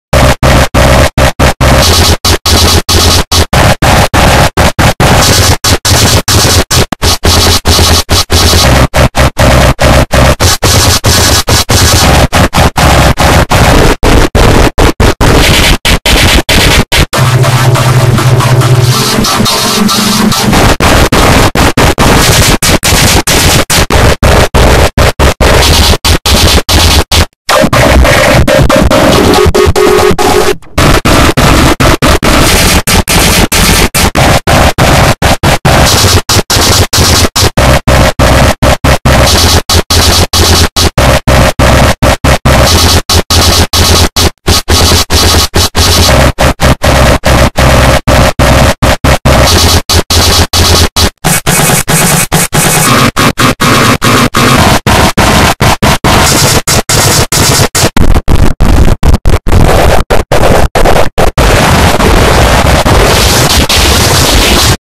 [REUPLOADED] Angry Preview 2 Effects (REFIXED)
Reuploaded again because it was set age restriction by community guidelines.

Original Description: Fixed from Bettie Conklin teh Object Thingy because she made it with VideoFX